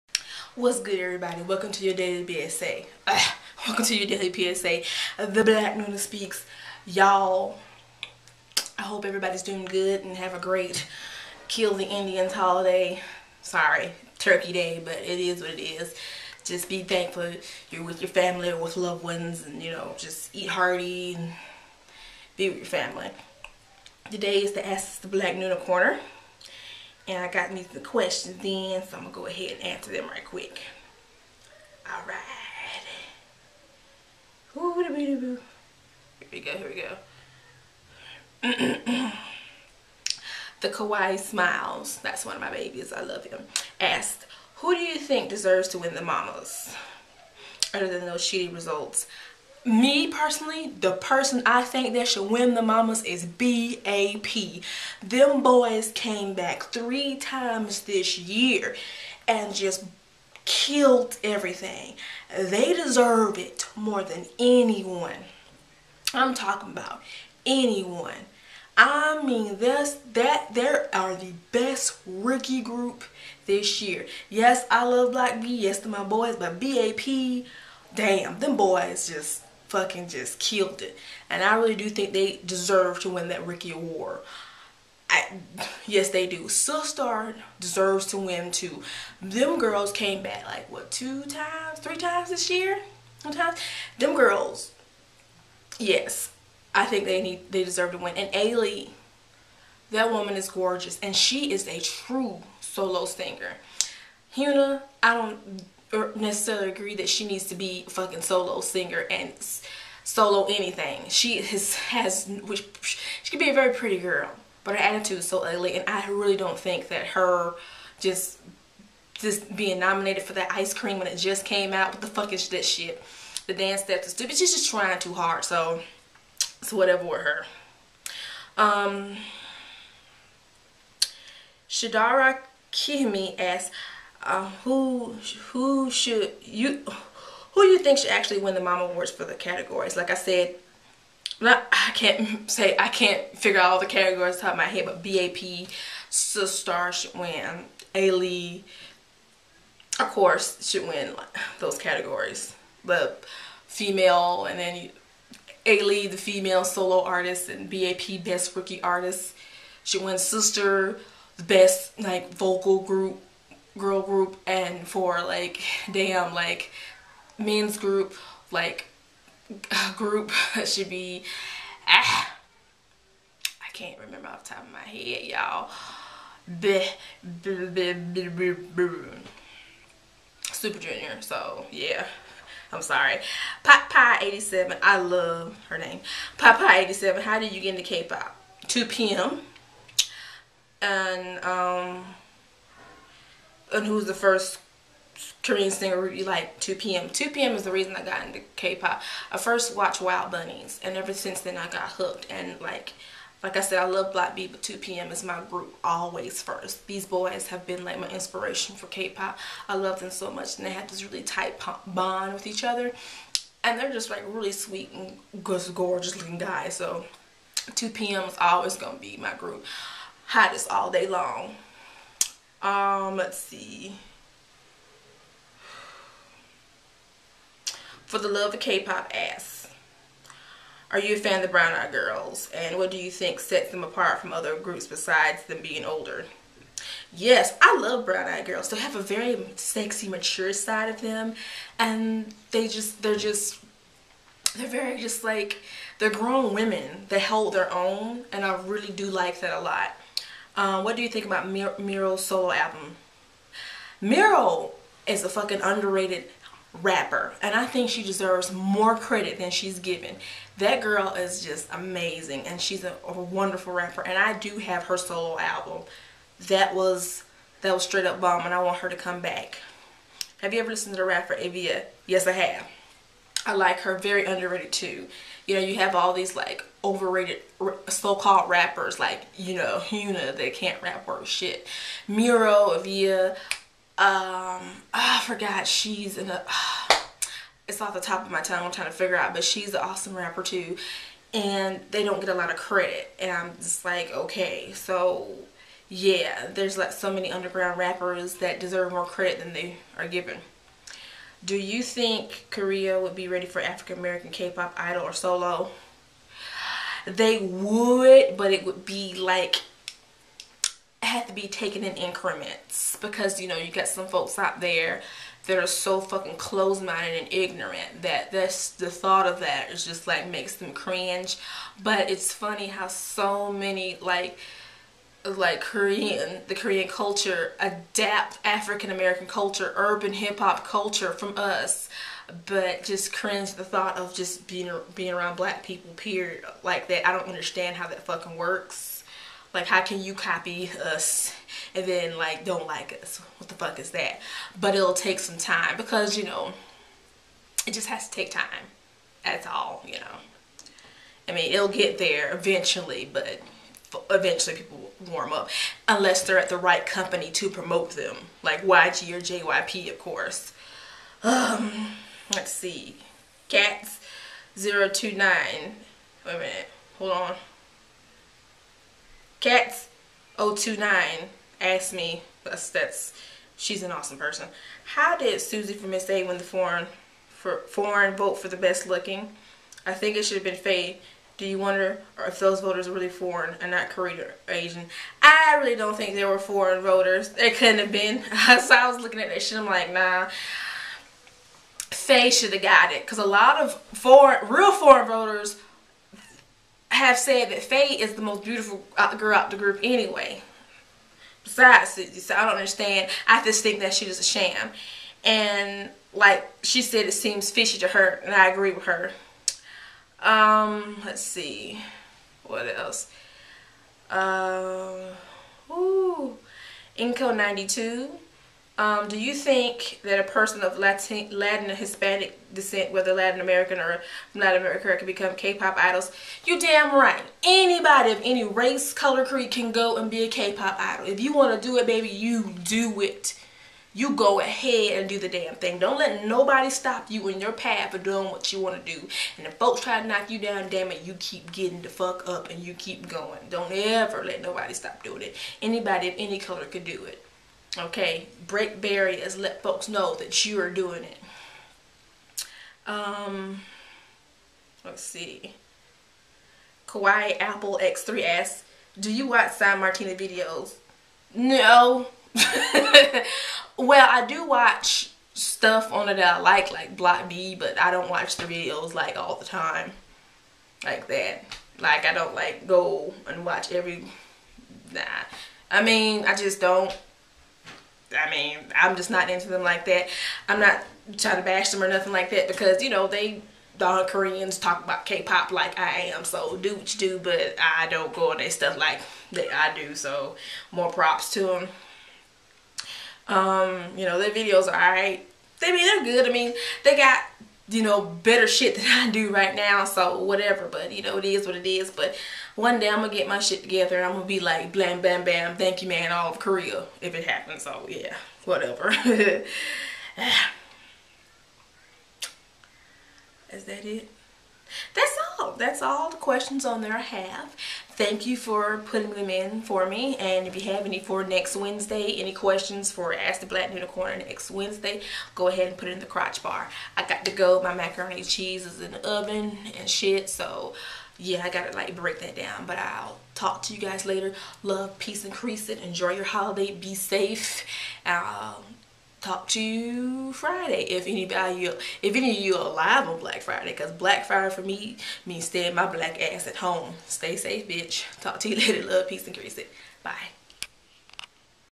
The Black Noona Speaks: Ask The Black Noona Corner Edition #4
Okay guys I'm back answerwing all of your KPOP, JPOP, and CPOP related questions this week...
So bring it on...

ShiBurrowes
Q: Do you think fans go too far with their love/obsession of OTPs? Like YunJae, YooSu, and other ships.
Q: Do you think that JYJ and TVXQ will ever get back together?

KawaiiApplex3
Q: Do you watch simon and martina's videos? and what do you think of their videos?

enko92
Q: Do you think a person of Latin/hispanic decent wether Latin American or from Latin America can become a kpop idol/s?

Catz029
Q: How did Suzy from Miss A win the foreign vote for best looking?? Do you wonder if those voters where really foreign voters and not Korean or any other asian ??

4TheLoveOfKPOP
Q: Are you a fan of Brown Eyed Girls and what do you think sets them apart from other girl groups besides them being older? Also what did you think About Miryo solo mini album this year?  Have you ever listened to the rapper E.via and if you have what do you think about her?  Do you think korea would be ready for an african american kpop idol or solo artist?

TheKawaiiSmiles
Q: Who do you think deserves to win at the MAMAs (other than those sh*tty "results")

potpie87
Q: How did u get into kpop? Like who was the first Korean singer or group that u liked?

Thank you guys so much for submitting your questions and be sure to submit your questions for next week's ASK THE BLACK NOONA CORNER...
You leave them all right here at the bottom of the crotch bar below...
Everyone be safe this holiday and get fat...